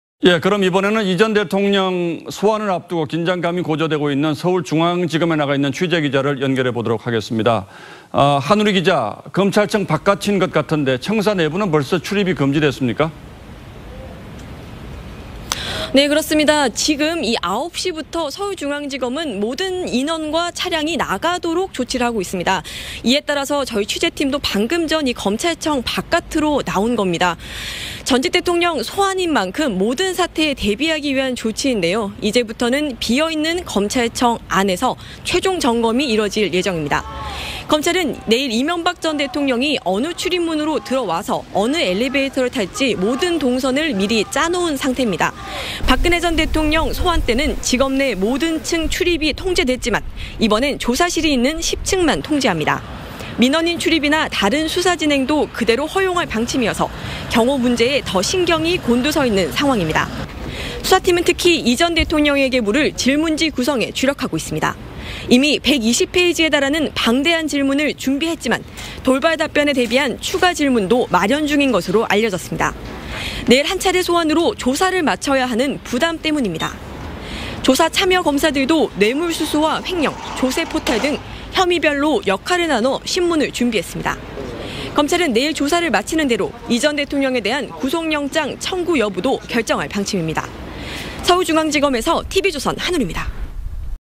[이 시각 서울중앙지검] 팽팽한 긴장감…조금 전부터 출입통제
그럼 이번에는 이 전 대통령을 소환을 앞두고 긴장감이 고조되고 있는 서울 중앙지검의 취재기자를 연결해 보겠습니다.

하누리 기자, 검찰청 바깥인 것 같은데 청사 내부는 벌써 출입이 금지됐습니까?